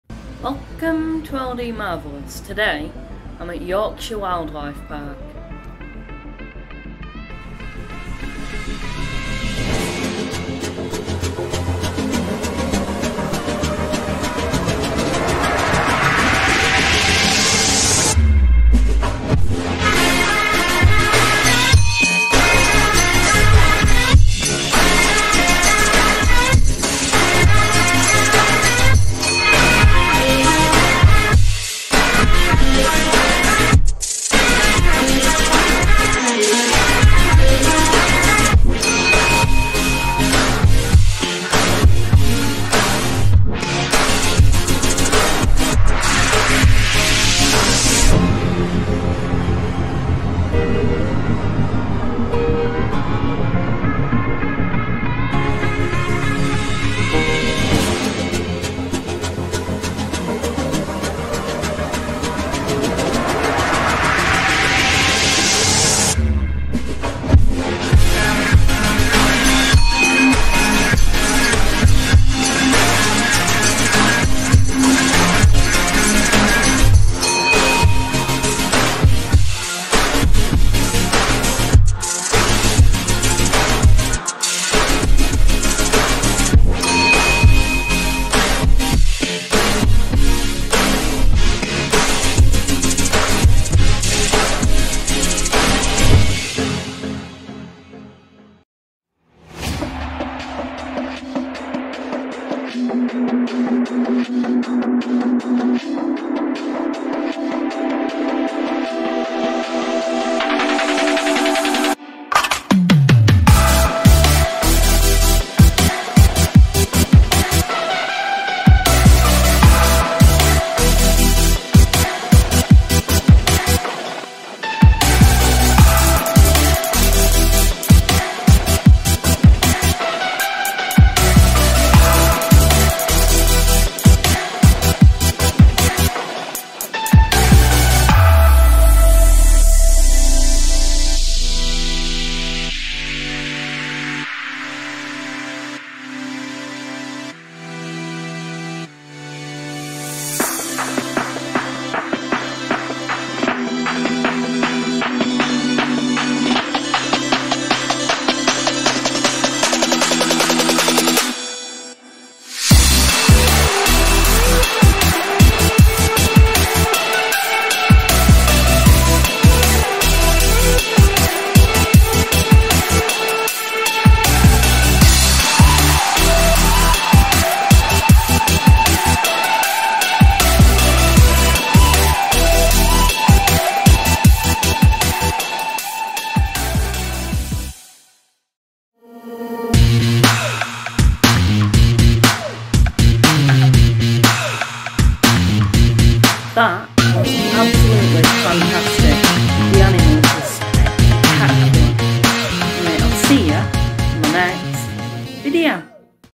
Yorkshire Wildlife Park
I came here on a school trip and it was great. This video was recorded on Tuesday 28th June 2022.